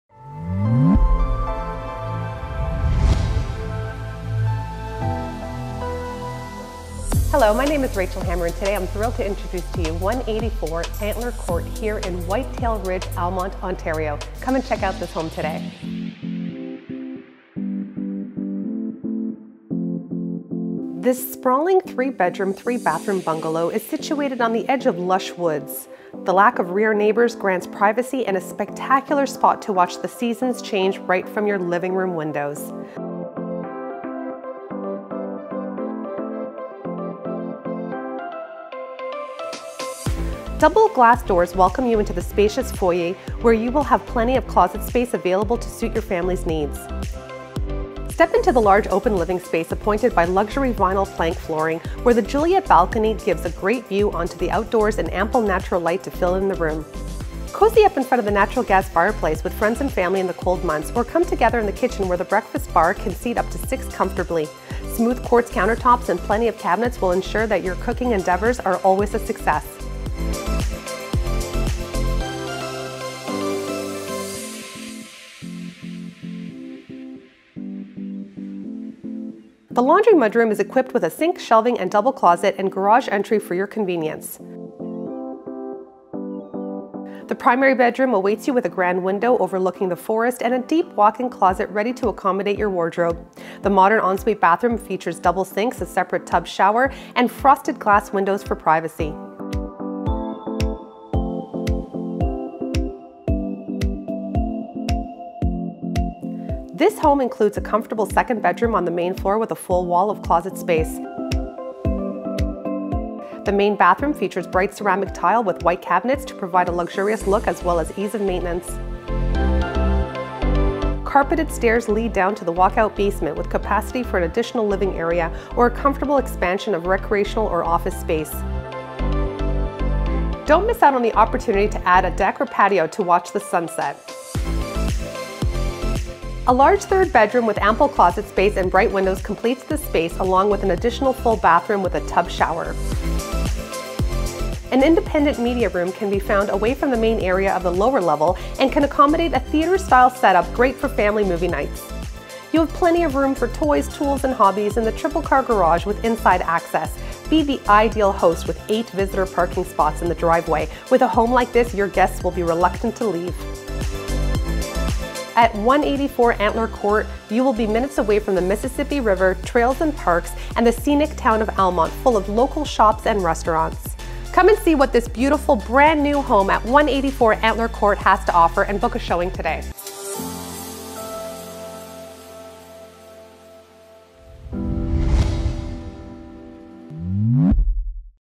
184 Antler Court - White Tail Ridge, Almonte - rachelhammer.com
Welcome to newly built 184 Antler Court in family friendly White Tail Ridge, Almonte. This sprawling three bedroom three bathroom bungalow is situated on the edge of lush woods, the lack of rear neighbours granting privacy and a spectacular spot to watch the seasons change right from your living room windows.

Double glass doors welcome you into the spacious foyer, where you will have plenty of closet space available to suit your family’s needs. Step into the large, open living space appointed by luxury vinyl plank flooring framed by upgraded baseboards and matching door/window casings. The Juliet balcony gives a great view onto the outdoors and ample natural light to fill the room.

Cozy up in front of the natural gas fireplace with friends and family in the cold months, or come together in the kitchen where the overhang island can seat up to six. Smooth quartz countertops and plenty of cabinets will ensure that your cooking endeavors are always a success.

Just a step away is the laundry/mud room equipped with a sink, shelving, a double closet, and garage entry for your convenience.

The primary bedroom awaits you with a grand window overlooking the forest and a deep walk-in closet ready to accommodate your wardrobe. The modern ensuite bathroom features double sinks, a separate tub and shower, and frosted glass windows for privacy.

This home includes a comfortable second bedroom on the main floor with a full wall of closet space, ready to welcome company. The main bathroom features bright ceramic tile with white cabinets to provide a luxurious look as well as ease of maintenance.

Carpeted stairs lead down to the fully finished walkout basement with capacity for an additional living area, or a comfortable expansion of recreational or office space. Don’t miss out on the opportunity to add a deck or patio to watch the sunset from through mature trees.

A large third bedroom with ample closet space and bright windows complete this space along with an additional full bathroom with a tub/shower.

An independent media room can be found away from the main area of the lower level and can accommodate a theater-style setup, great for family movie nights.

You have plenty of room for toys, tools and hobbies in the triple car garage with inside access. Be the ideal host with eight visitor parking spots in the driveway – with a home like this, your guests will be reluctant to leave.

At 184 Antler Court, you will be minutes away from the Mississippi River, trails and parks, and the scenic town of Almonte full of local shops and restaurants. Take advantage of the nearby Blakeney Rapids for a summer picnic, or sit back and watch the timeless Mill Falls on Mississippi.

Come see what the lush life at 184 Antler Court has to offer, and book a showing today.